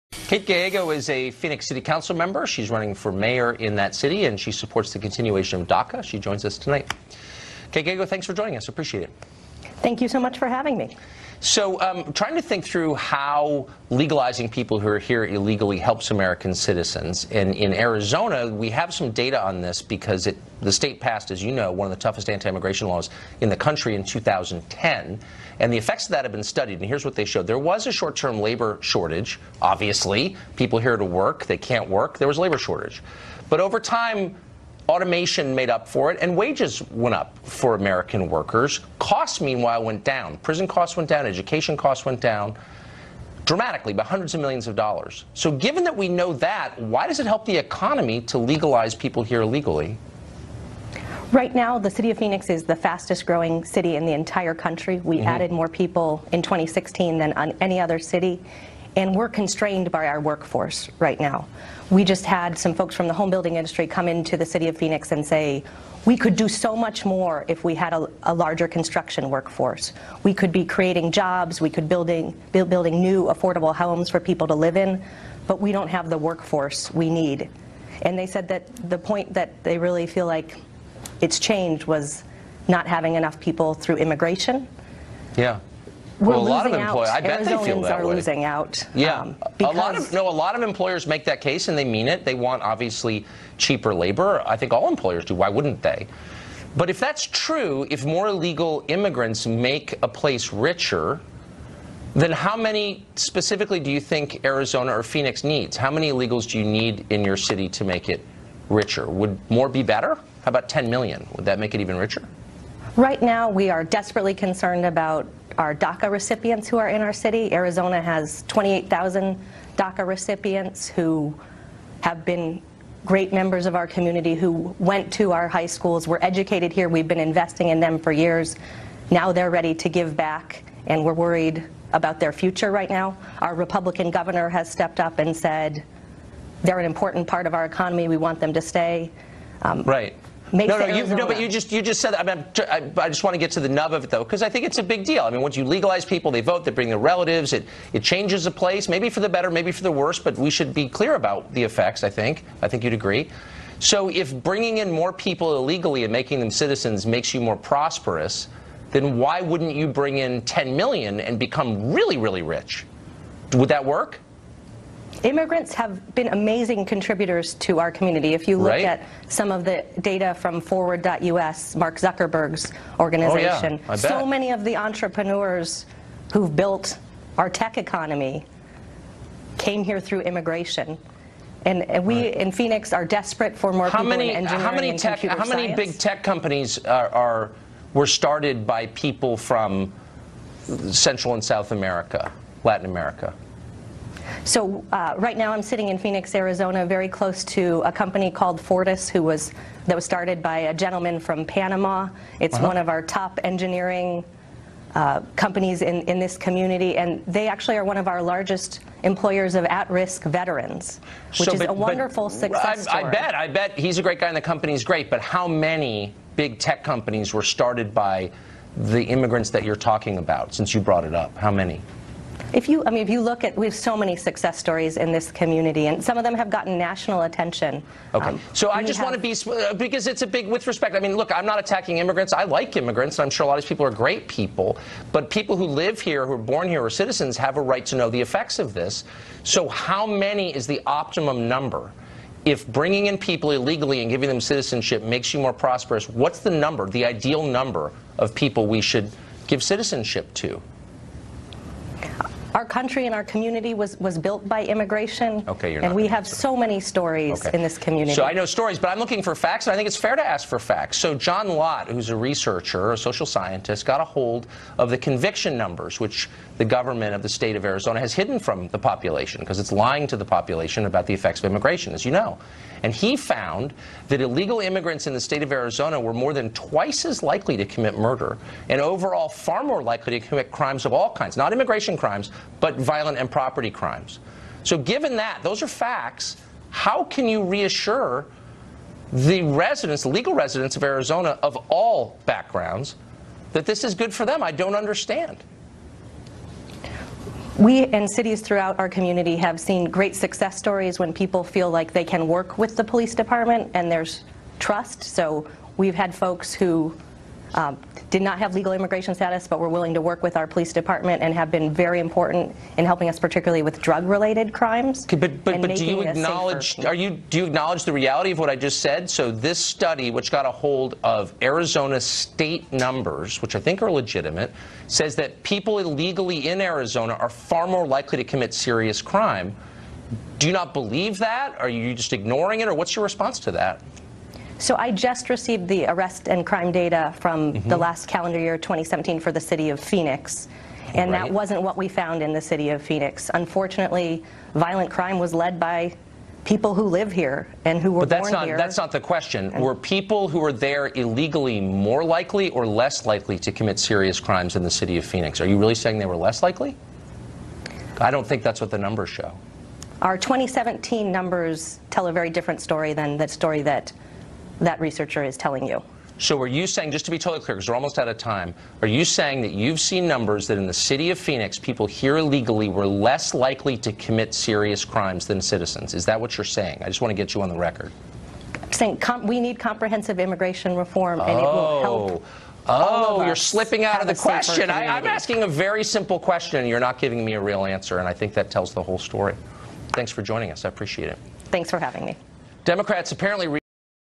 Does Diversity Make Us Stronger?
Tucker Carlson interviews Phoenix City Councilor asking "if mass immigration is so good, why don't we allow more in?"